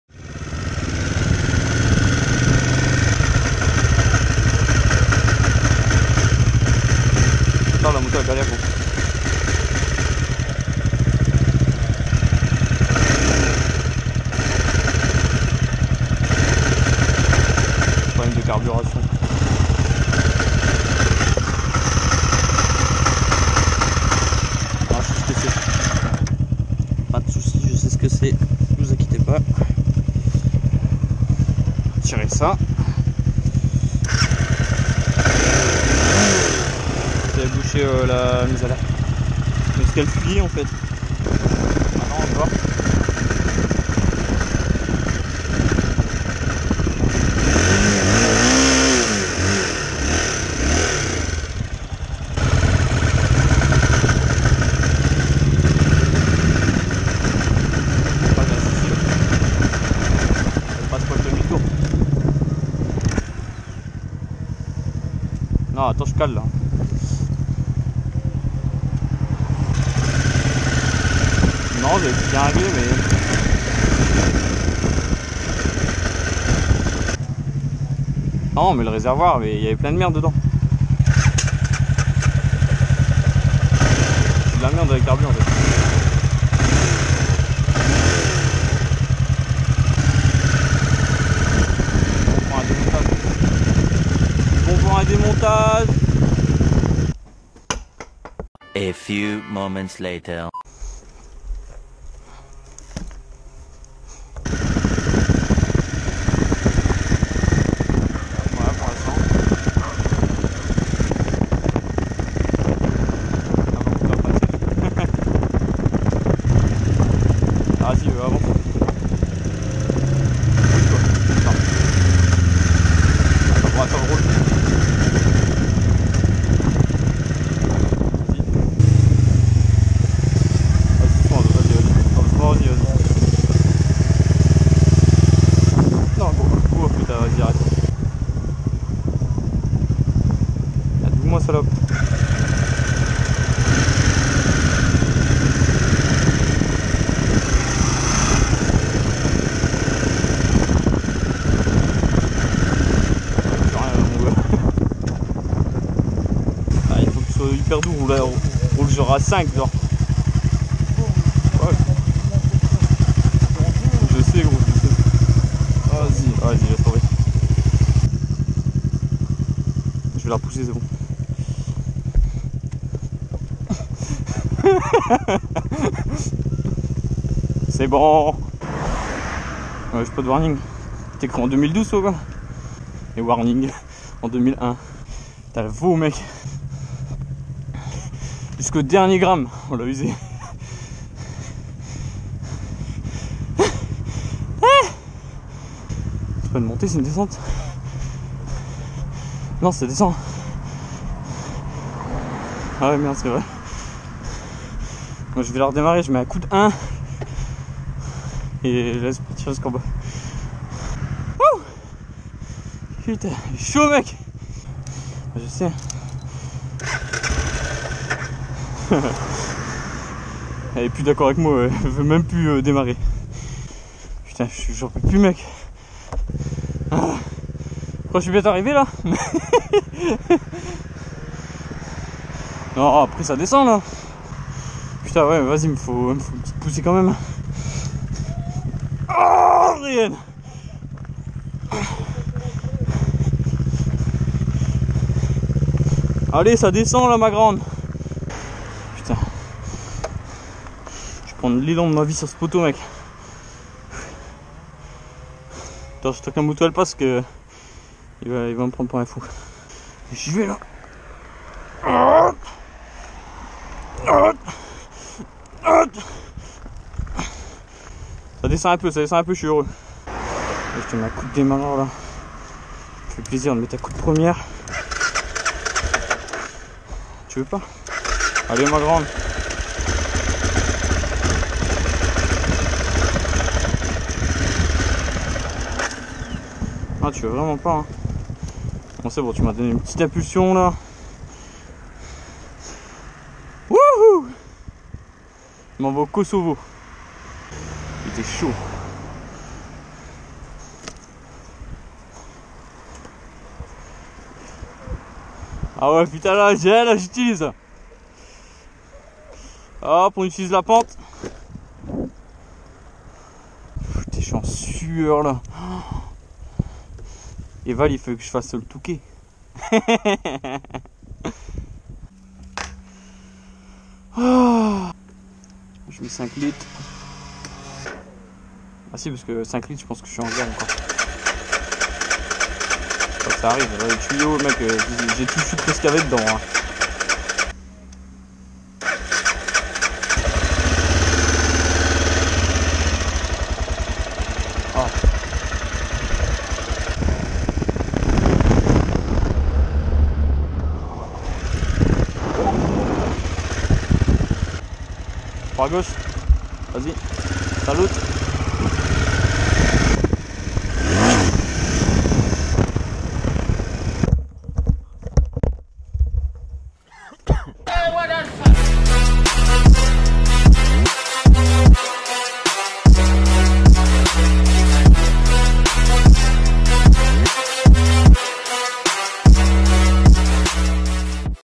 PETIT PROBLÈME D'ESSENCE
Ça partait pourtant bien ! mais la balade fut écourtée a cause d'essence pas très clean que j'ai remis dans le réservoir quelques jours auparavant, heureusement avec l'aide de mon acolyte on a vider tout ça pour aller en remettre de la belle toute jolie a la pompe..